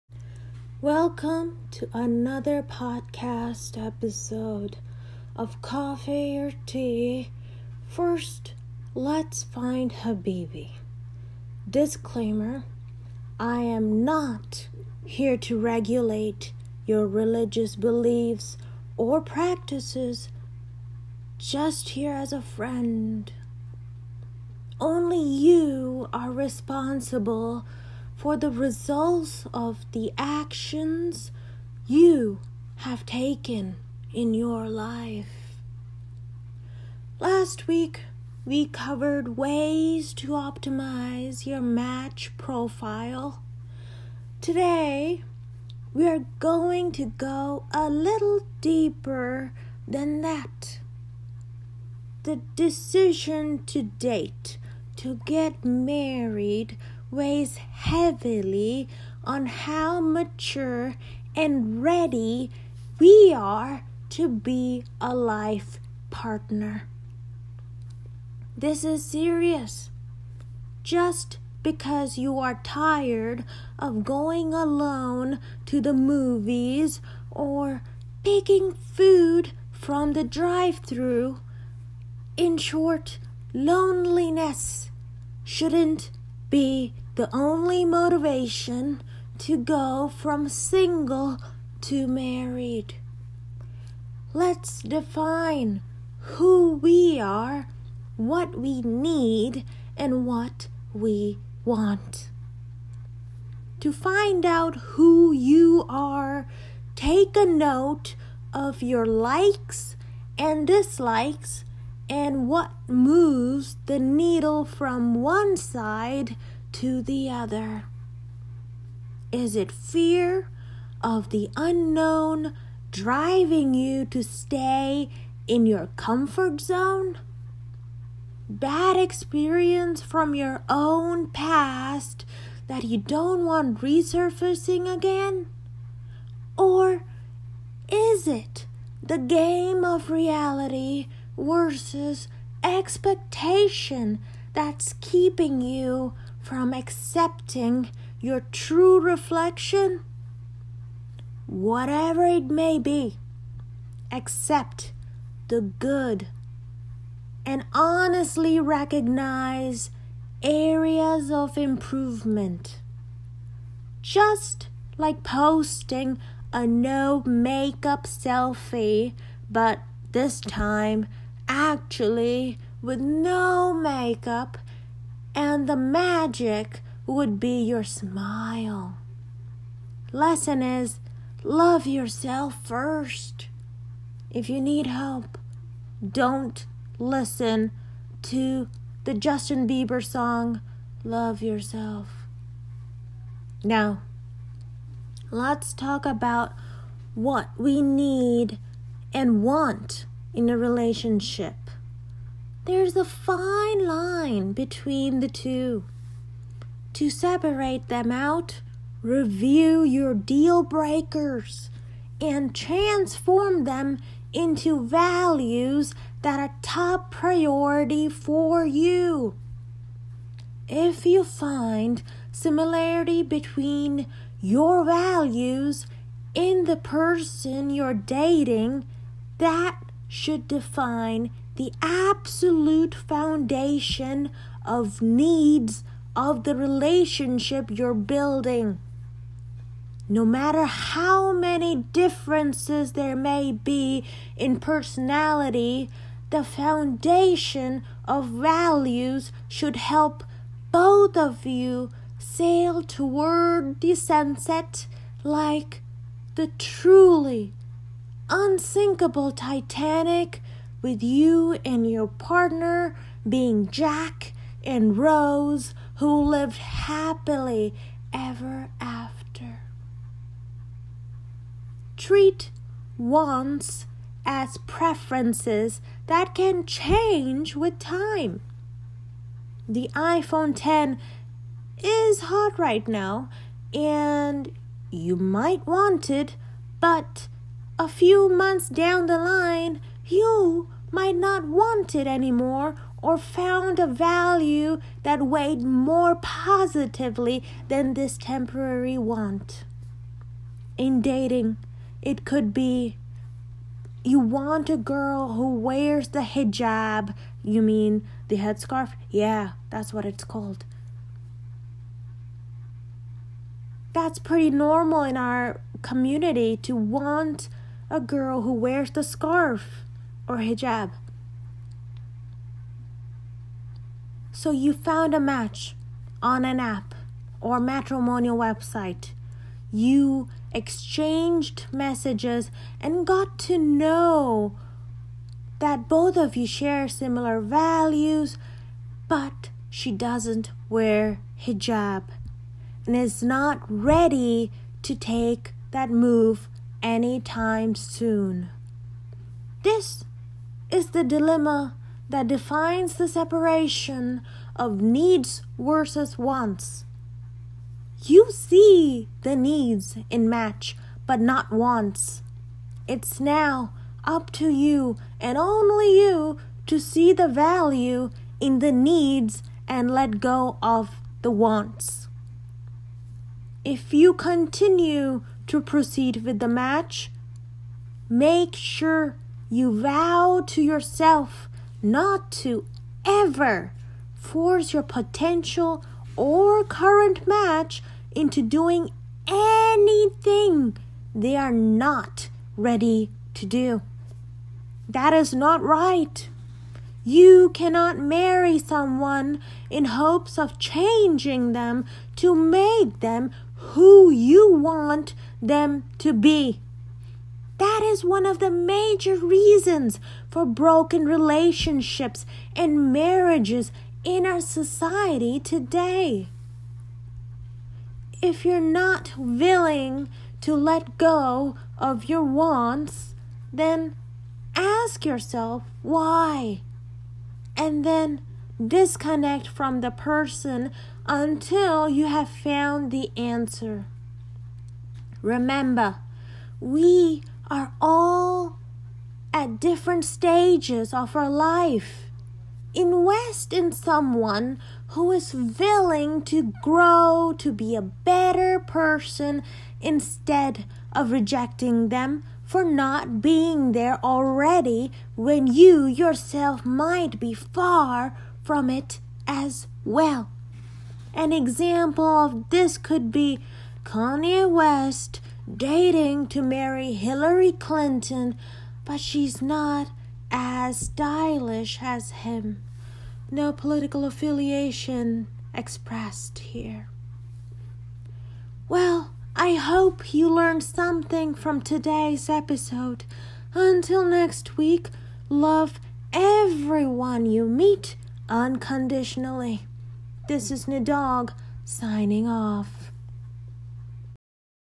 Coffee or Tea? First Lets Find habibi Podcast Episode 4 by Nidawgh
LISTEN to Podcast Episode 4 of Coffee or Tea? First Lets Find habibi to learn the difference between what we need and what we want, and realize the importance of who we are!

About:
Coffee or Tea? First Lets Find habibi is a Podcast by Muslim Indian American comedian Nidawgh. The podcast episodes will encompass dating tips some meant to be taken seriously while others to just laugh out loud.